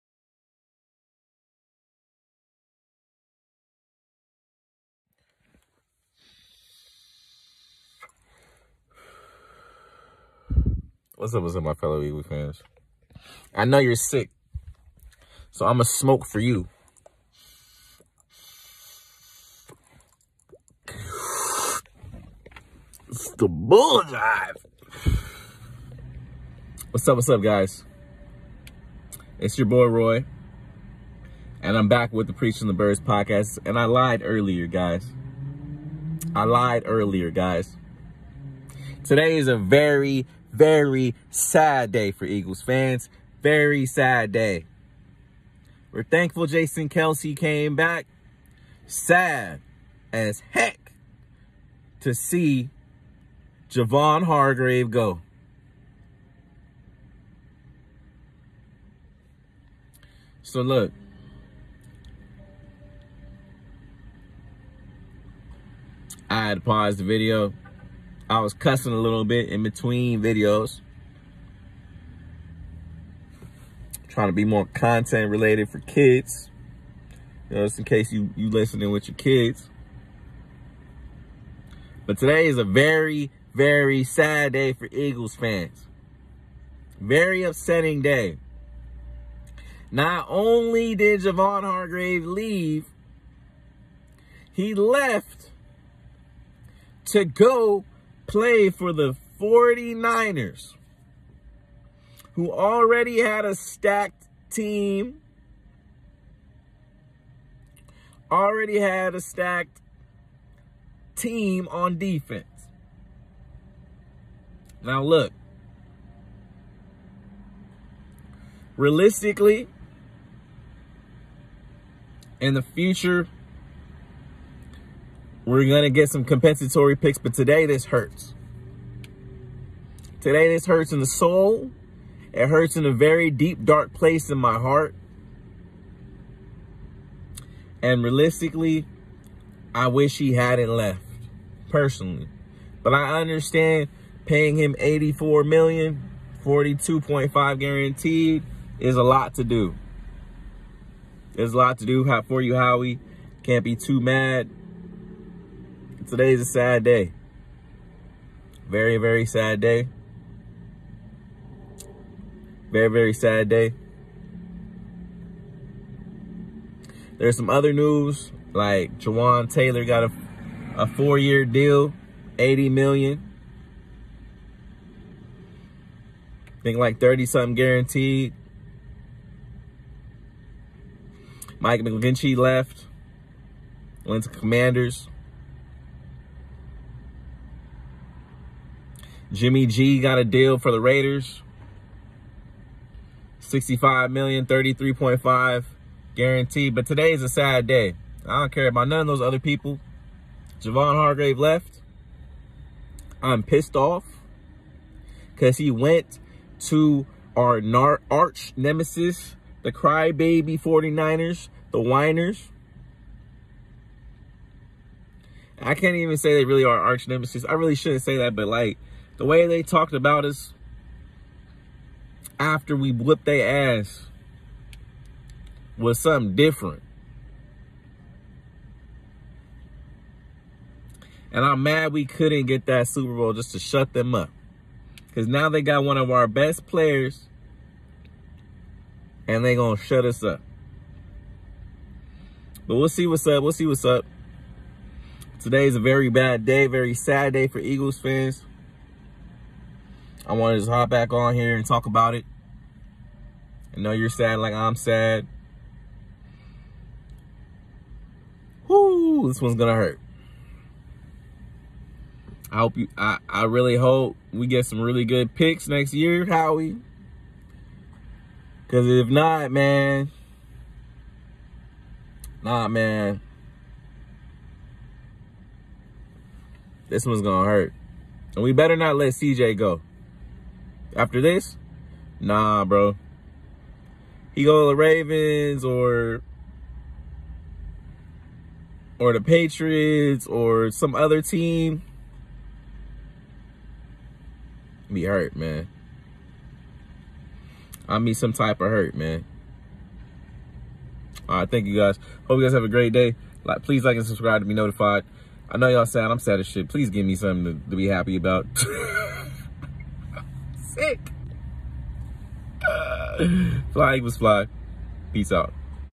Javon Hargrave to the 40 Niners means a sad day for us eagles fans. In Howie we trust but why there?
Today on the @Preachingtothebirdspodcast we discuss the sad news that Javon Hargrave is leaving us for the 49ers. We also talk about other minor news related to the free agency  but mainly the sad news that Javon is leaving us